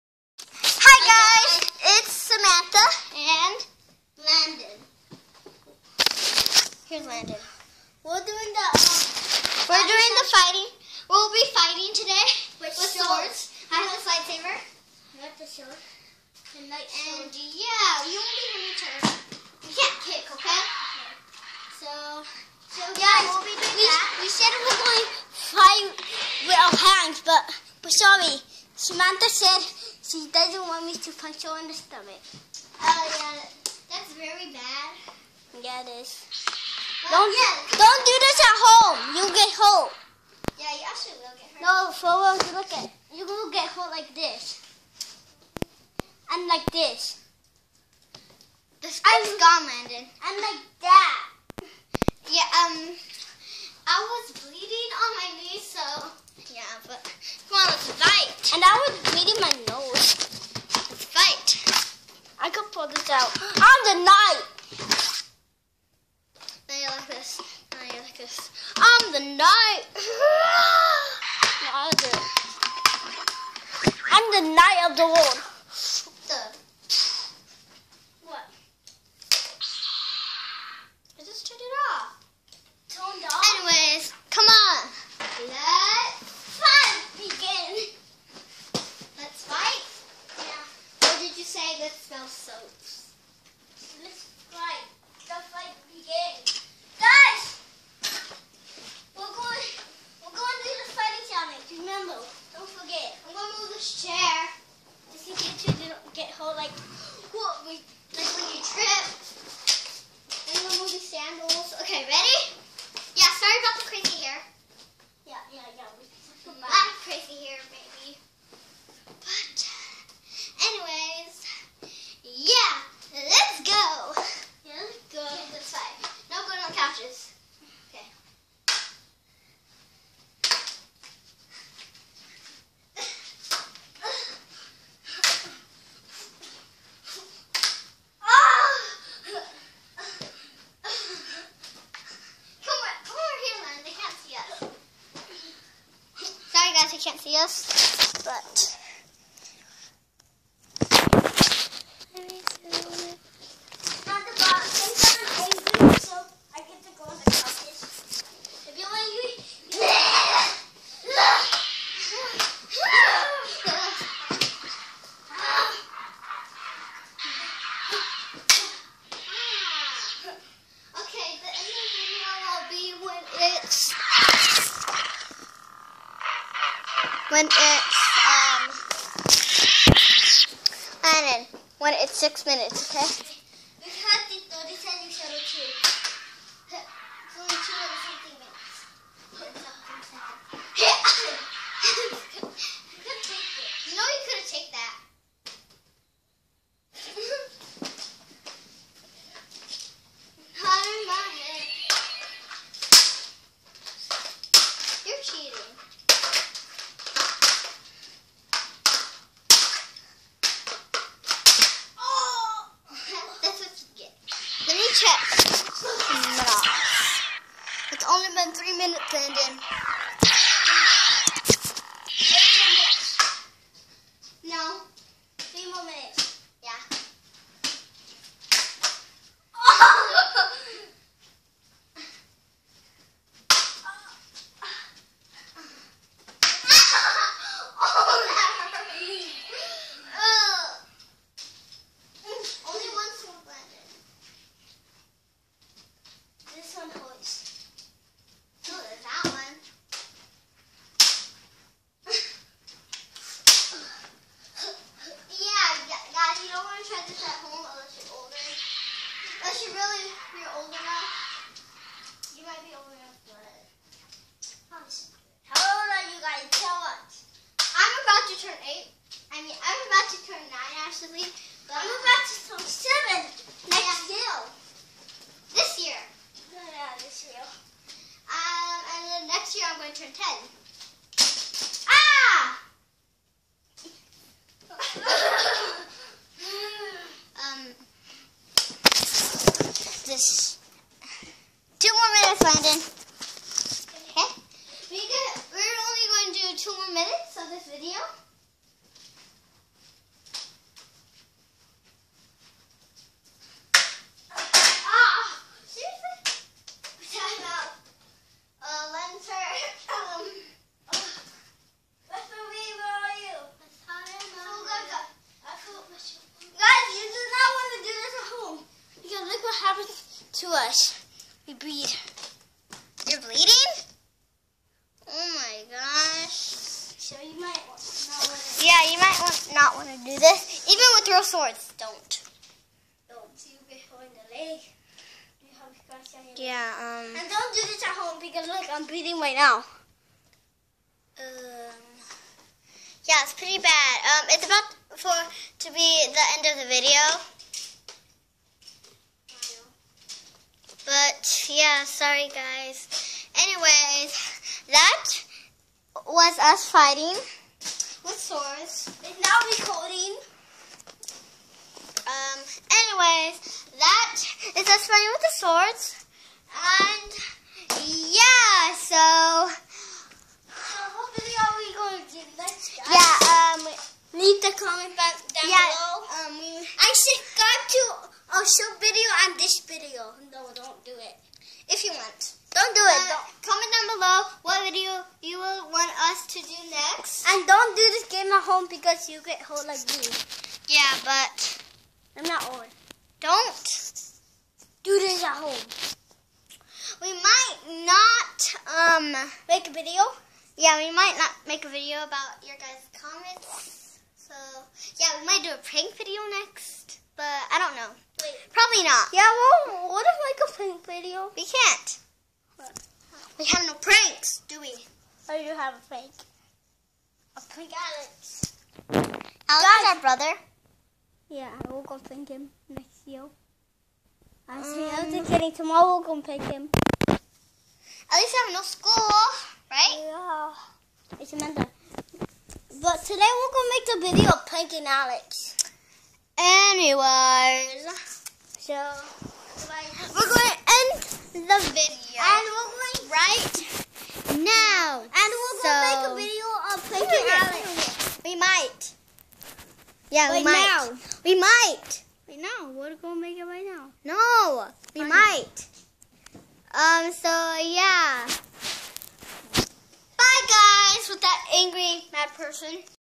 Fighting with swords
Hi guys you don't really want to do this at home cause you can bleed like my brother anyway bye guys